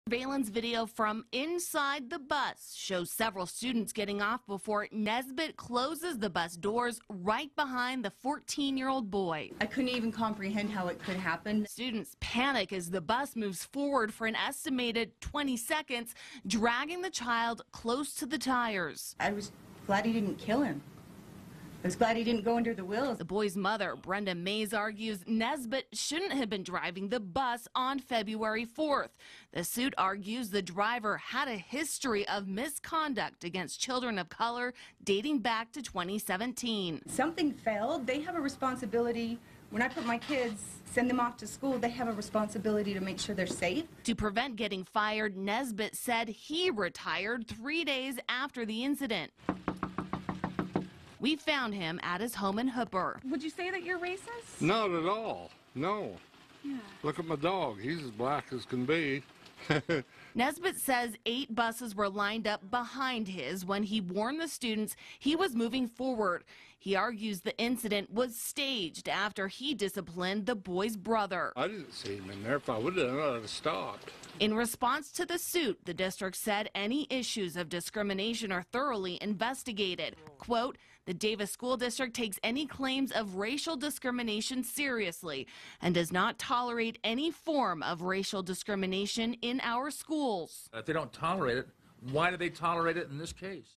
School bus driver accused of intentionally dragging student
The incident took place on February 4 of this year in front of West Point Junior High School in Davis County, Utah. The 14-year-old boy, a seventh grader at the school, was one of the students being taken to class that morning. As the bus pulled into the school’s front driveway at 7:59am, the boy is seen filing out of the bus with his classmates. But when the boy reaches the last step before exiting the bus, the driver, John Naisbitt (inset), apparently places his hand on a lever used to open and close the door. The door shuts with the boy’s backpack on the inside of the bus and the boy’s body dangling outside, dangerously close to the bus’s front right tire.